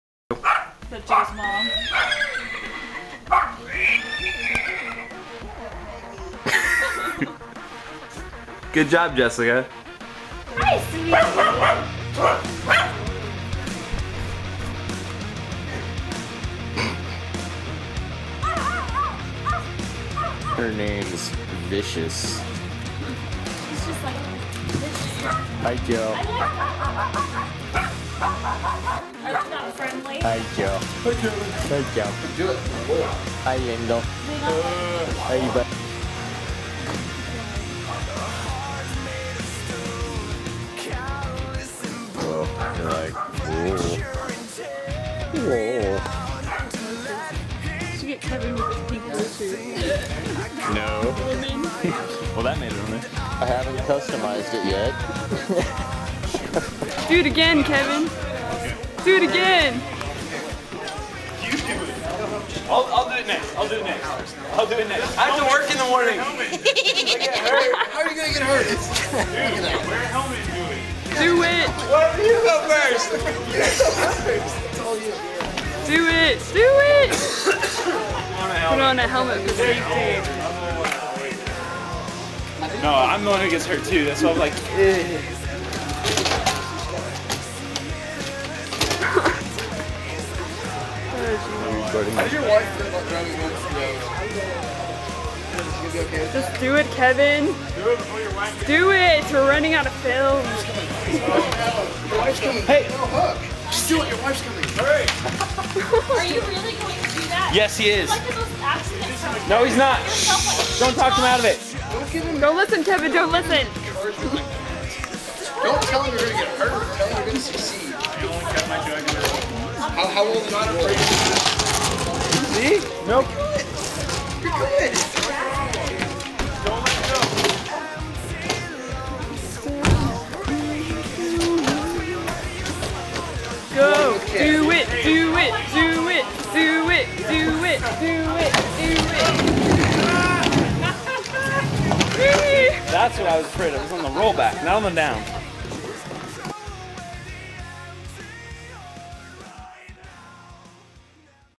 Vicious and the Medford Vandals
This is a random video of some stuff we did before and after our concert in Medford, Oregon